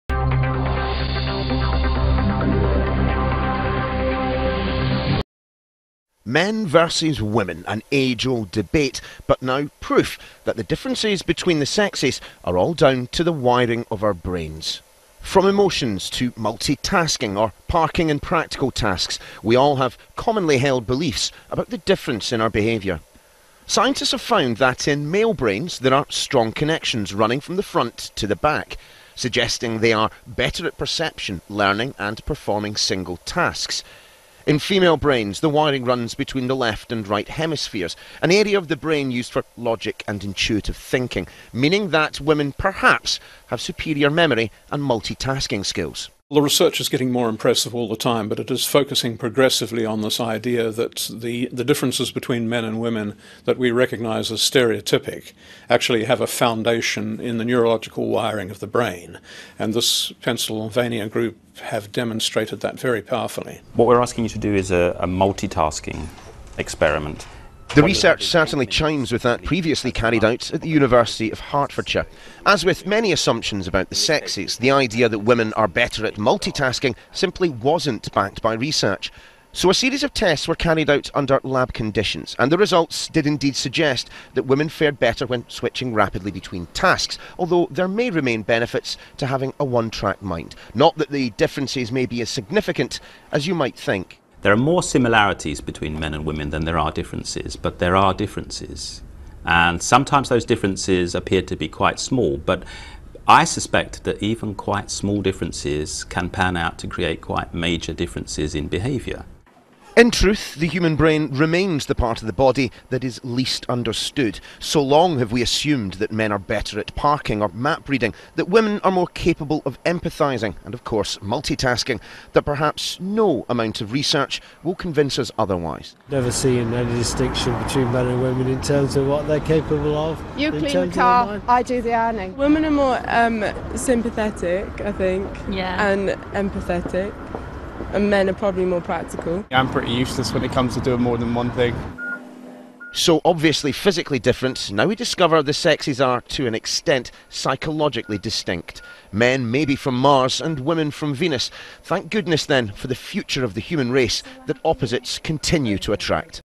Brain study shows why men and women are different
A new study looking at the neural wiring of the male and female brain has concluded that there is a greater degree of neural connectivity from front to back within one hemisphere in males, suggesting brains were wired to facilitate connectivity between perception and co-ordinated action. Women's brains meanwhile are wired between left and right hemispheres, indicating they facilitated communication between the analytical and intuition and are better at multi-tasking.